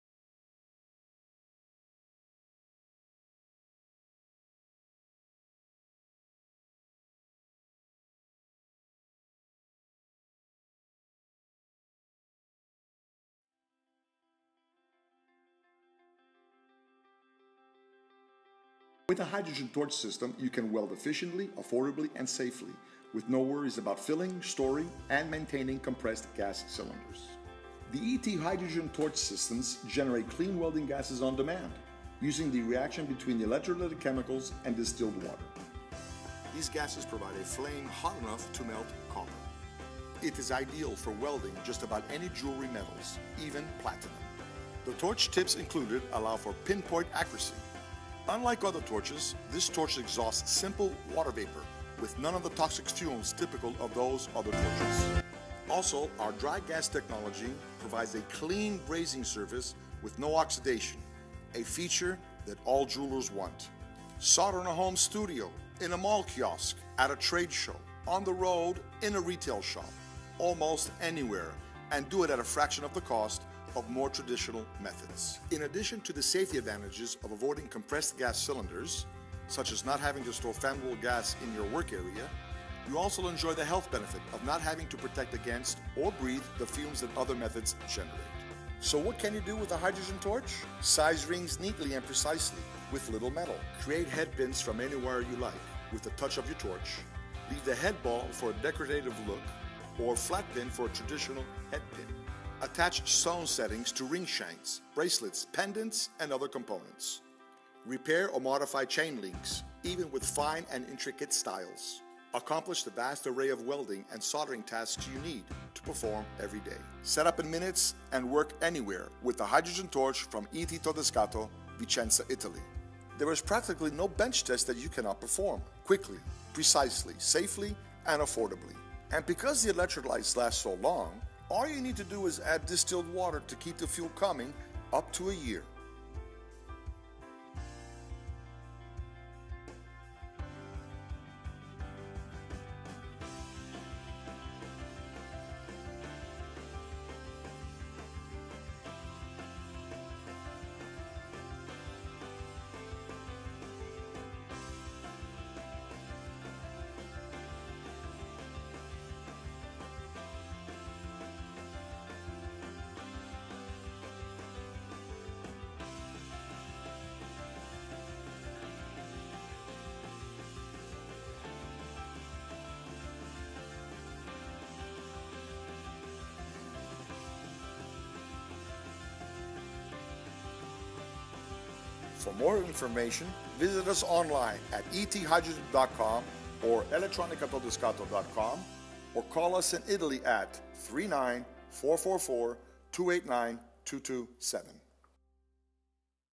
PRESENTING ET HYDROGEN TORCH
In this video from ET hydrogen, take a short tour that highlights several of the benefits you can get using a hydrogen torch. See tasks such as creating head pins, re-tipping prongs, resizing rings and more. And the benefits go beyond the work you can do. Working with a hydrogen torch, you never have to deal with bulky, dangerous, expensive tanks of compressed gas. And the cast-off from your soldering is simple water vapour, not potentially health-threatening fumes.

E.T.  stands for makers who create with their hands and their hearts and who are courageous enough to make jewellery their livelihood.